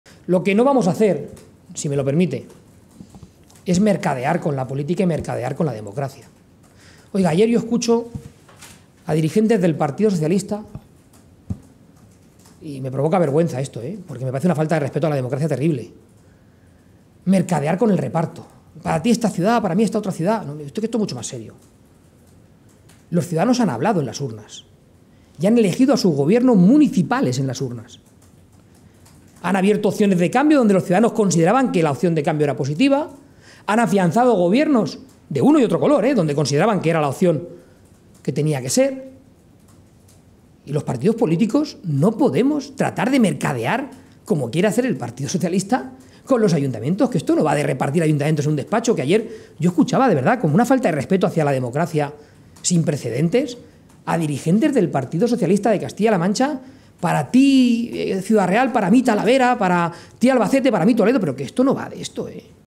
Núñez acusa al PSOE C-LM de "mercadear" con la democracia
El presidente del PP en Castilla-La Mancha, Paco Núñez, ha acusado al PSOE de "mercadear" con los ayuntamientos cuando insiste en pedir a los 'populares' respetar la lista más votada en las principales ciudades. "Es una falta de respeto a la democracia sin precedentes", ha sentenciado el líder de los 'populares' en la región.